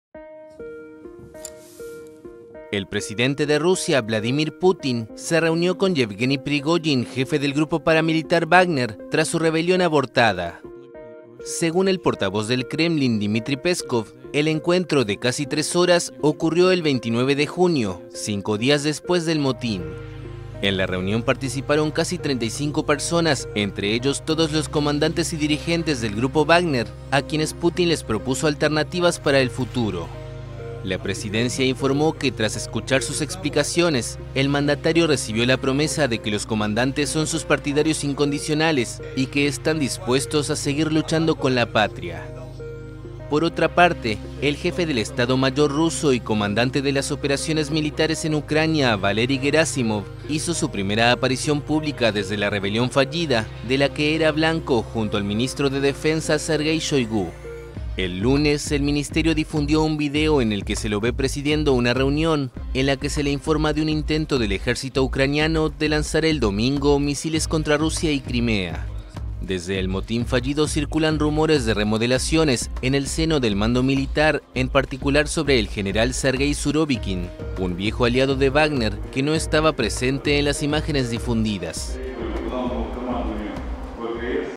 🚨 PUTIN se reunió con JEFE DEL GRUPO WAGNER tras la rebelión, según el Kremlin
🚨 PUTIN se reunió con PRIGOHZIN, JEFE DEL GRUPO WAGNER, tras la rebelión, según el Kremlin

El presidente Vladimir Putin se reunió el 29 de junio en el Kremlin con el jefe del grupo paramilitar ruso Wagner, Yevgueni Prigozhin, cinco días después de su rebelión abortada, anunció el lunes la presidencia rusa.